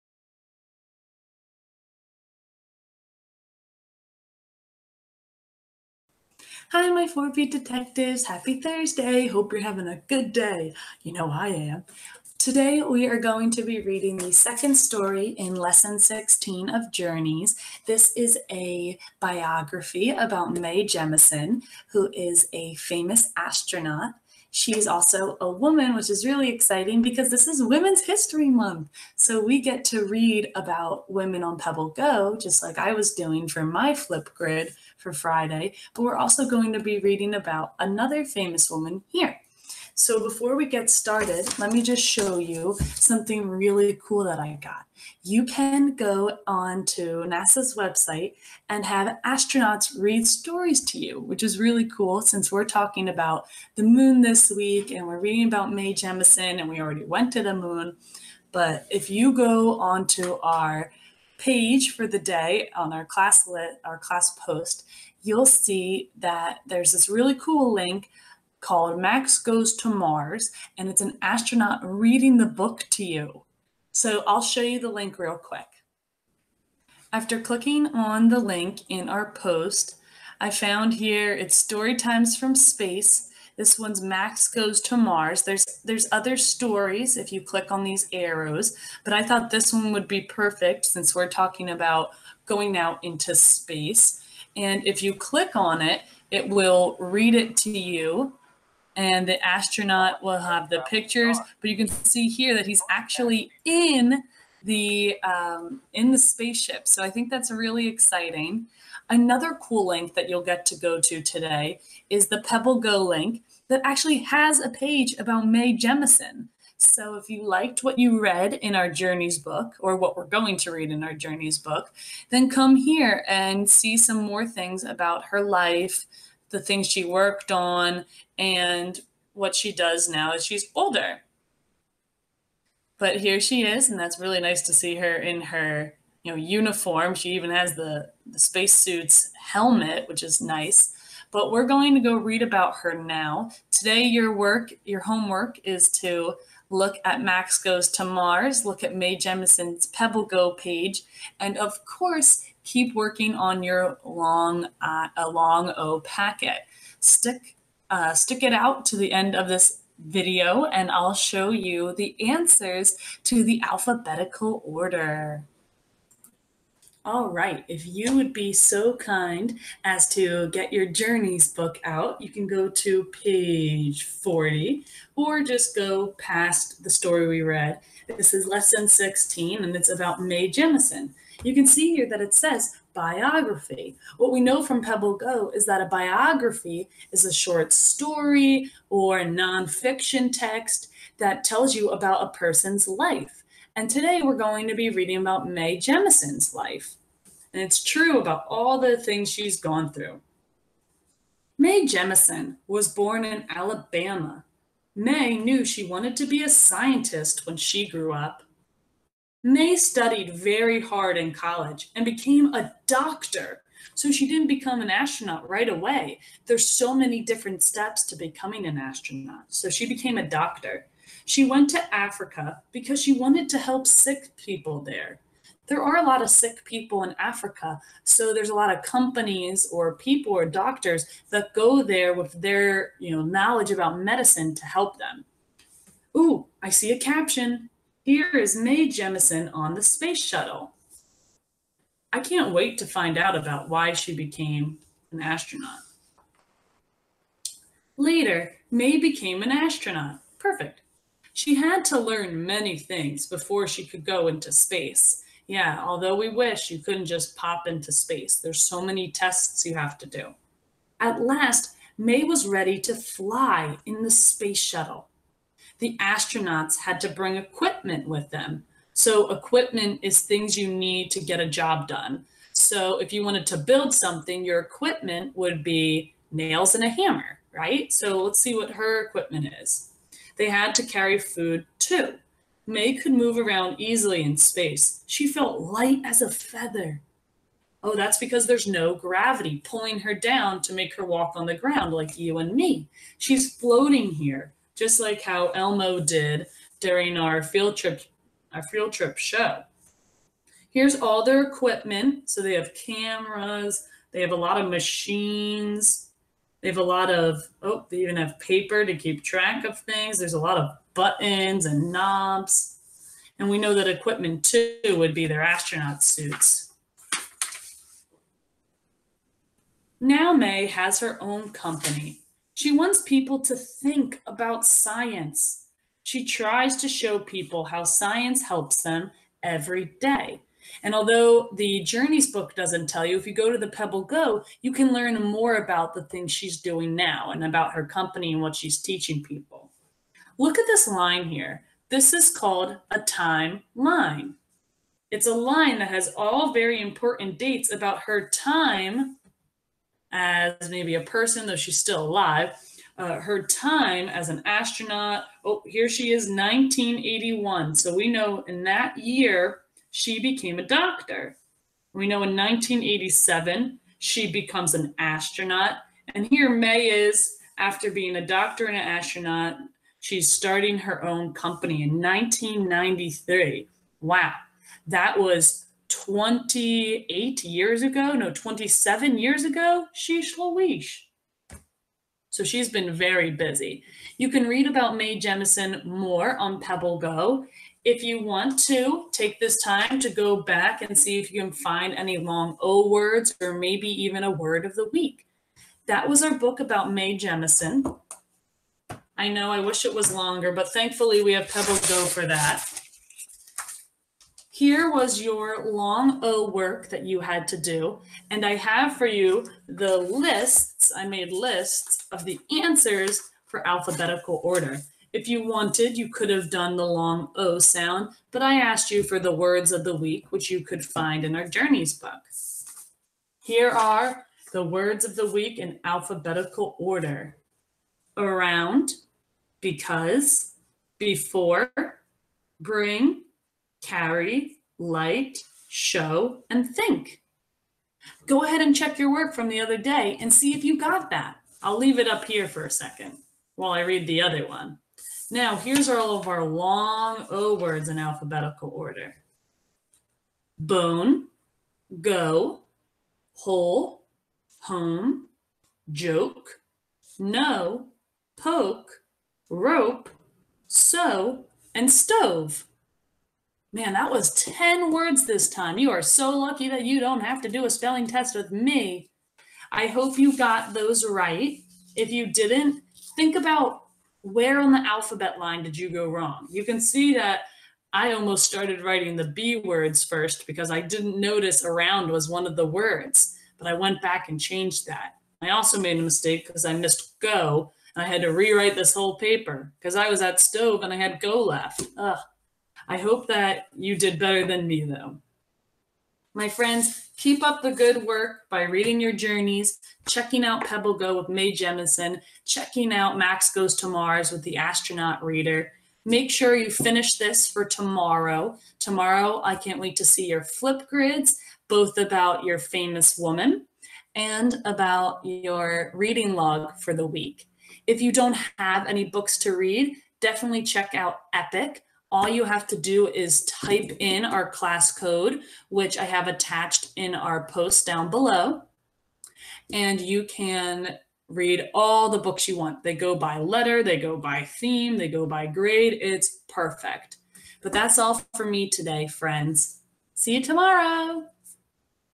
Journeys Lesson 16 Mae Jemison
Hi, detectives! Today we are continuing our Journeys lesson about the moon by learning about a famous female astronaut! Have you ever heard about Mae Jemison before? Well check out the biography in our books about her, and check out the two cool links I posted about both her and NASA's read alouds!!